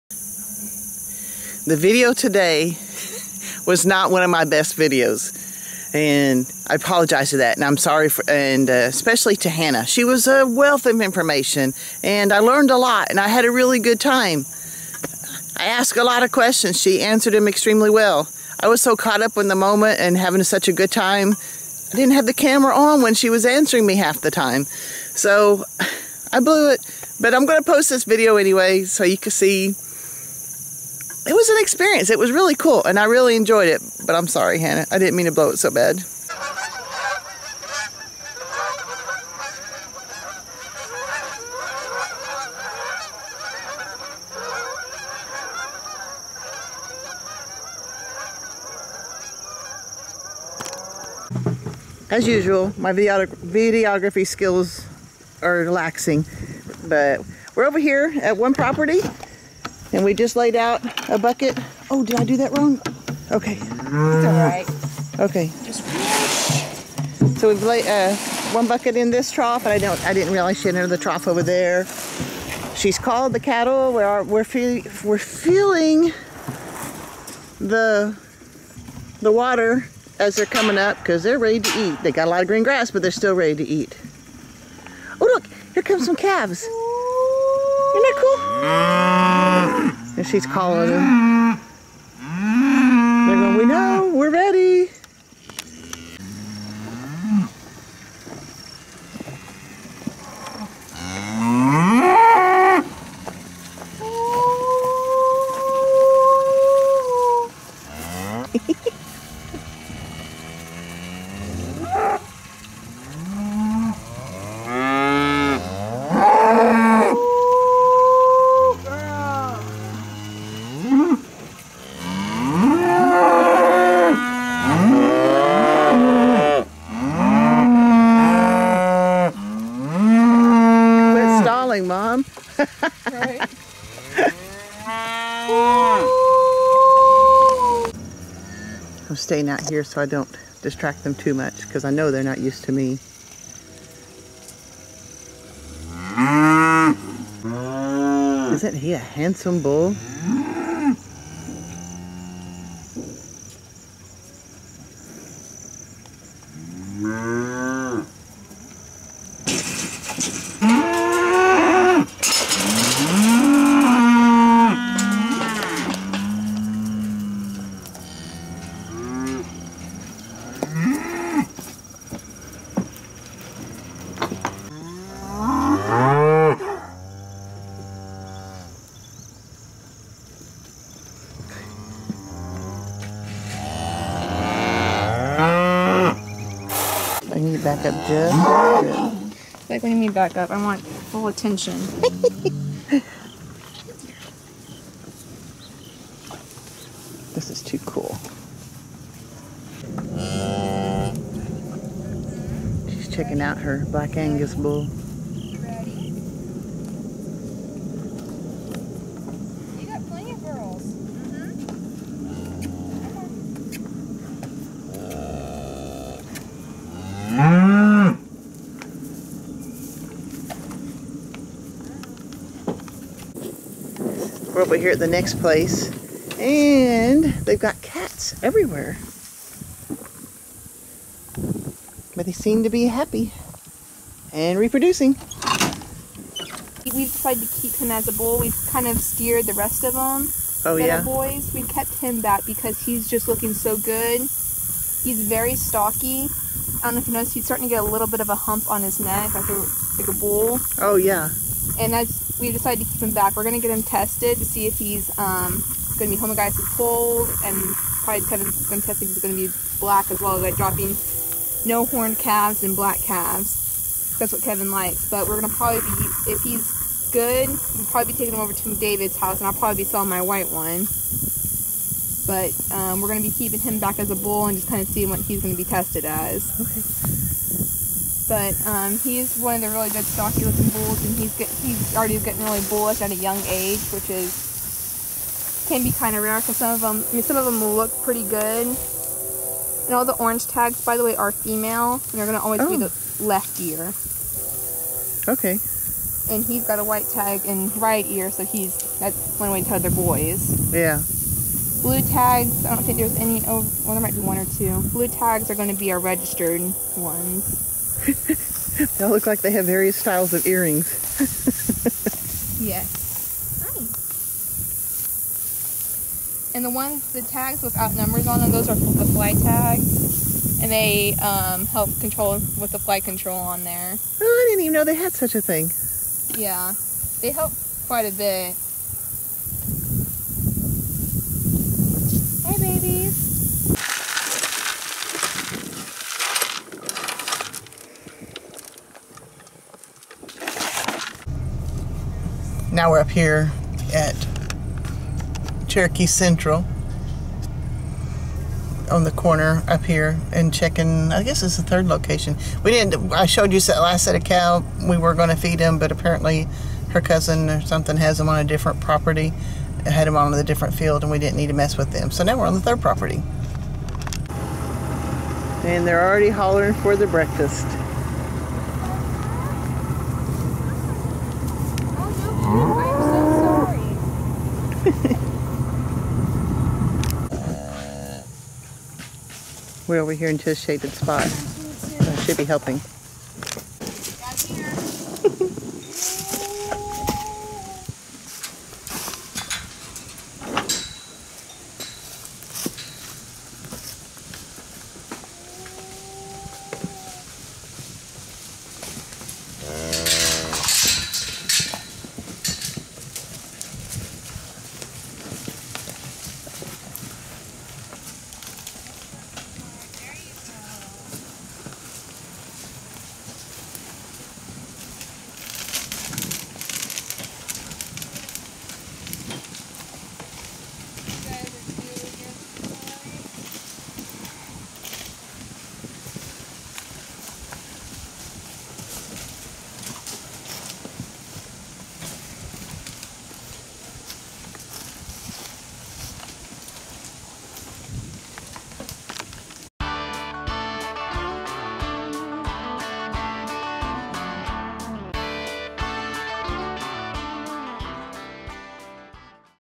I Get to Work Cattle With a Friend
I've been wanting to learn a little about cattle for a long time, as I consider what I want to put on our property to graze.

Hannah lets me tag along as she checks and feeds their herds of cattle.

Etsy/Kinzie's Candle Shop